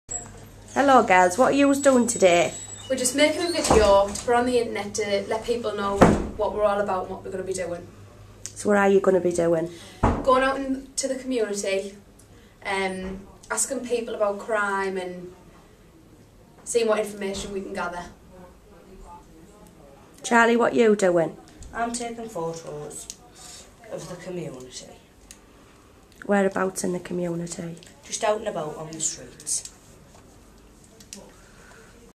Charlie and Martine
This is charlie and martine giving you a breif interview on what we are about.